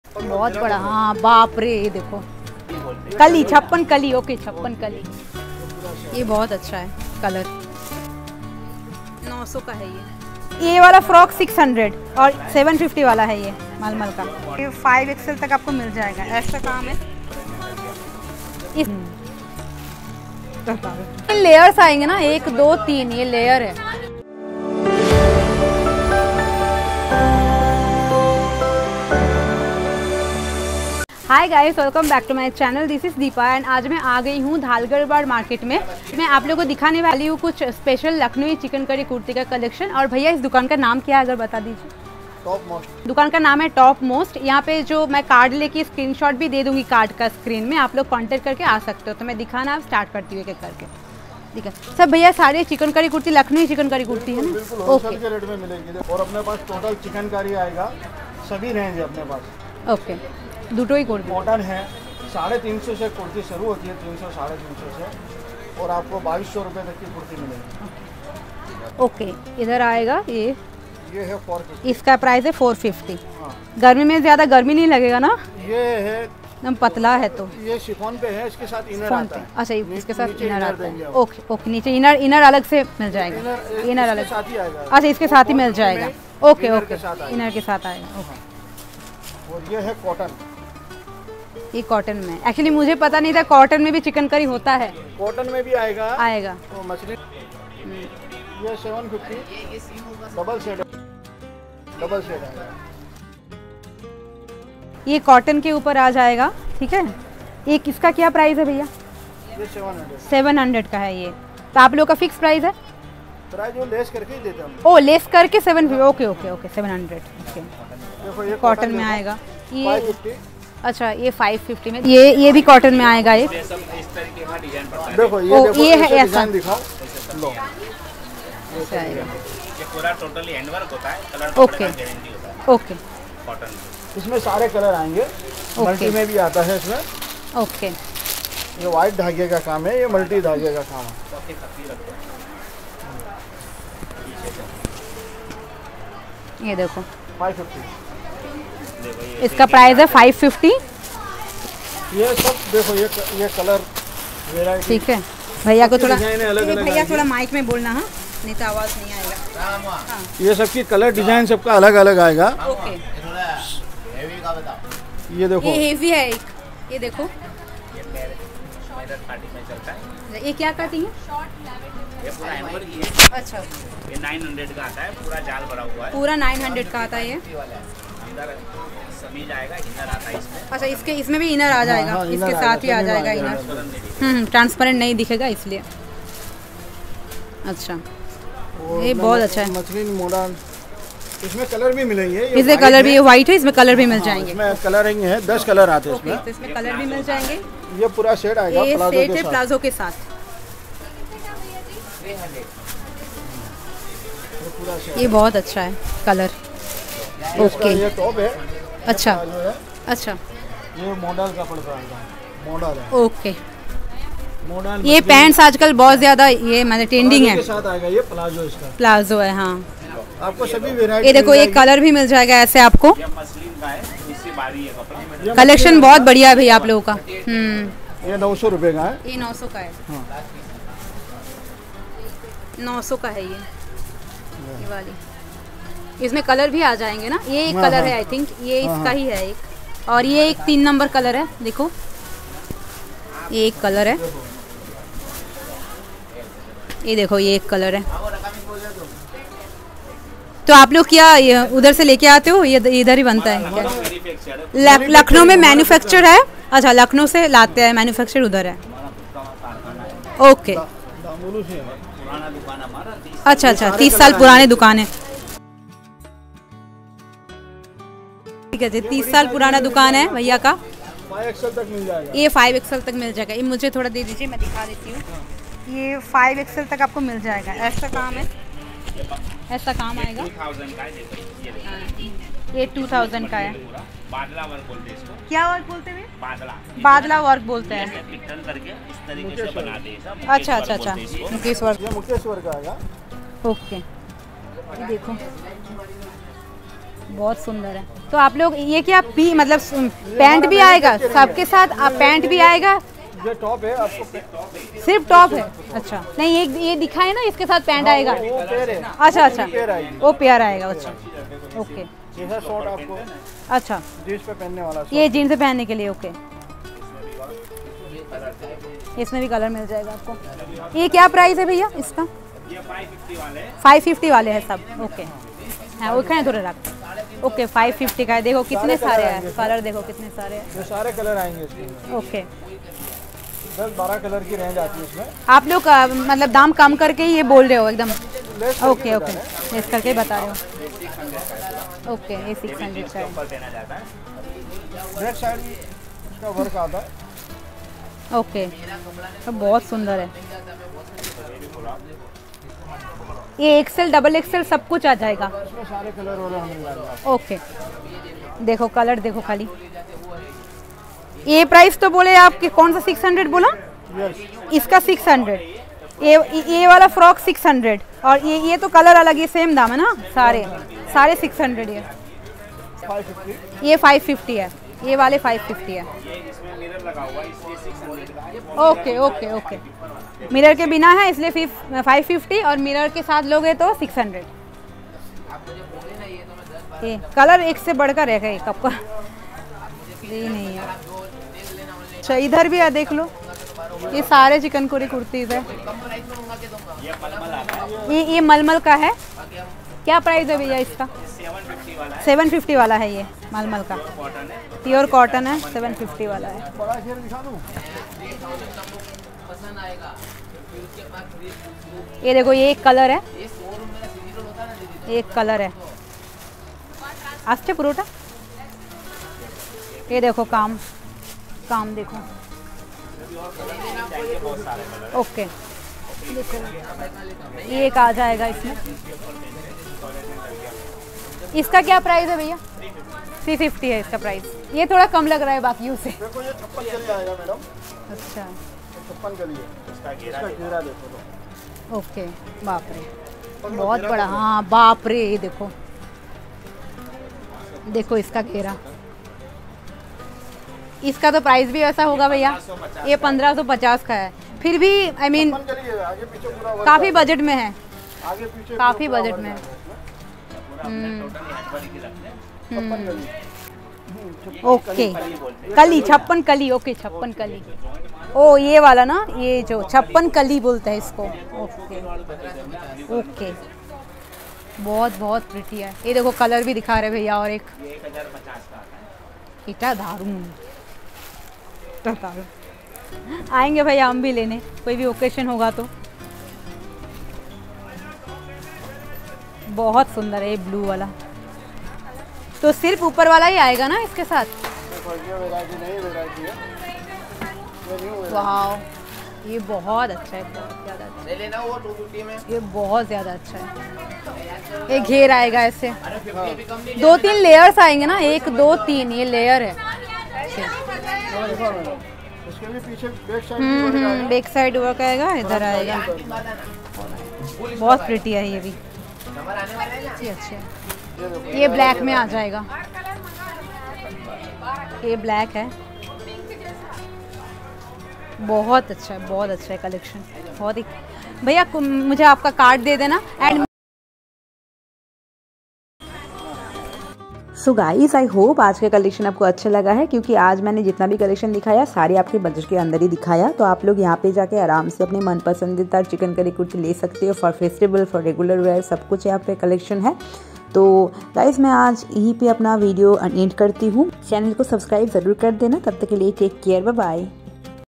Cheapest Lakhnavi Chikankari Kurti in Ahmedabad l Long kurti only from ₹ 350 l Street shopping l
Cheapest Lakhnavi Chikankari Kurti in Ahmedabad l Long kurti from ₹ 350 l Street Shopping l

Shop name : Top Most
Dhalgarwad Kapad Bazar, Ahmedabad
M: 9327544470